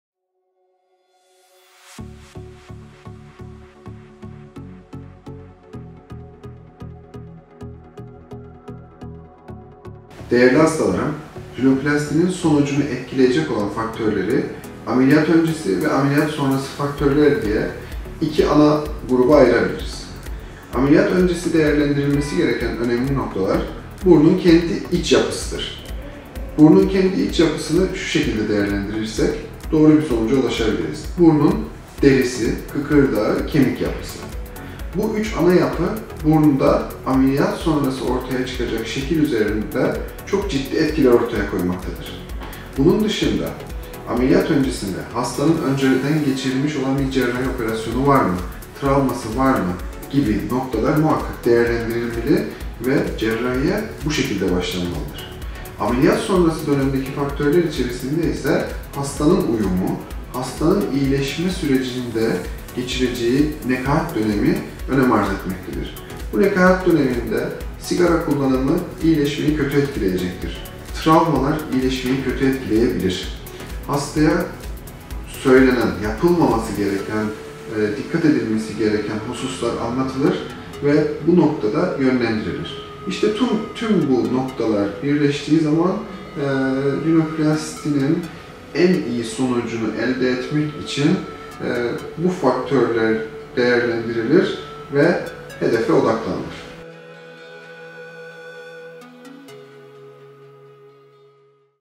Rinoplasti sonucunu neler etkiler?
Op. Dr. Hüseyin Kandulu, "Rinoplasti sonucunu neler etkiler?" sorusunu yanıtlıyor.